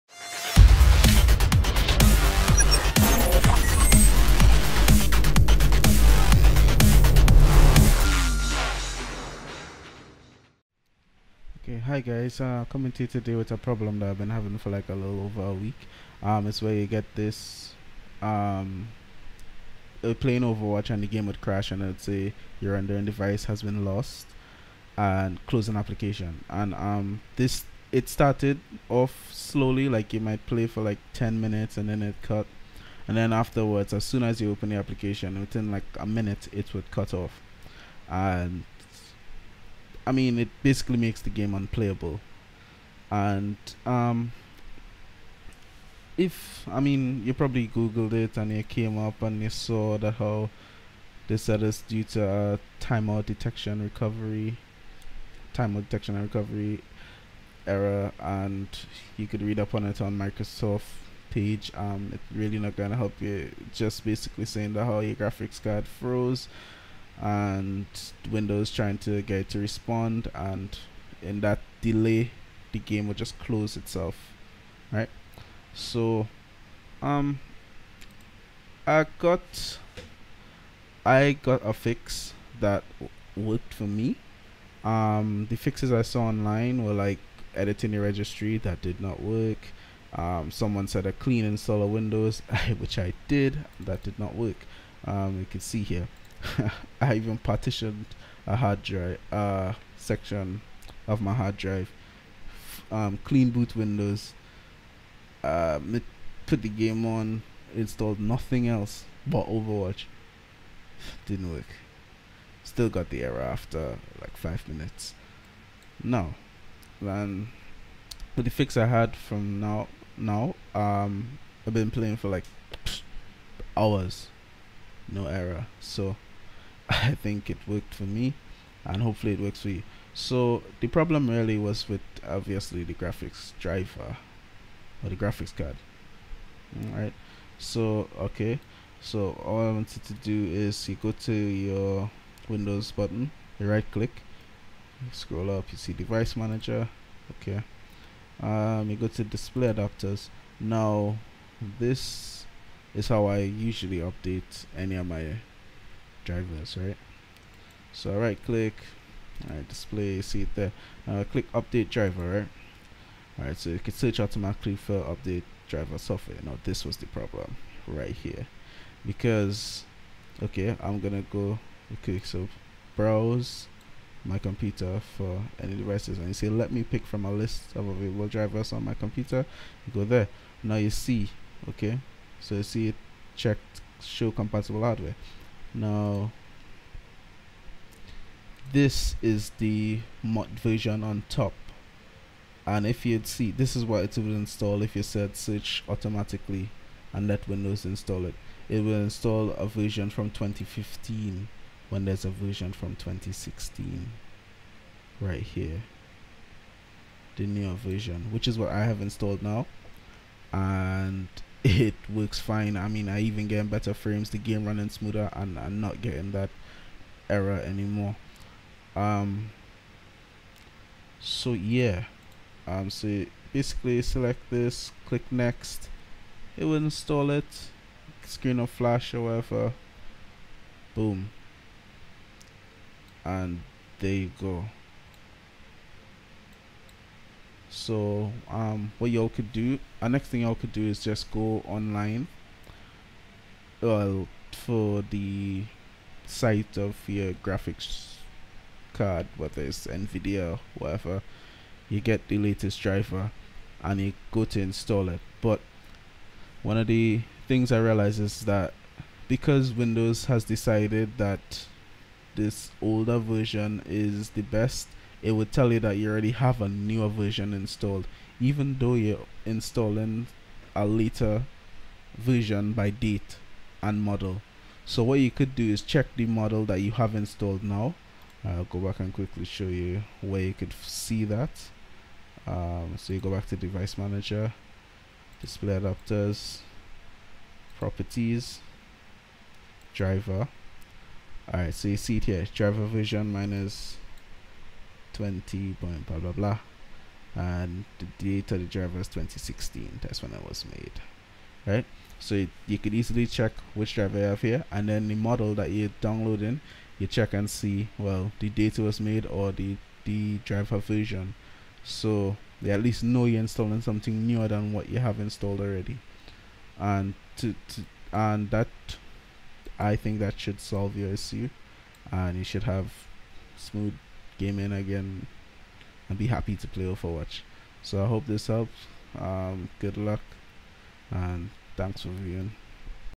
Your Rendering Device Has Been Lost - Overwatch Error Fix Intel HD
Hope this helps if you encounter this problem, it  works for me as of June 2017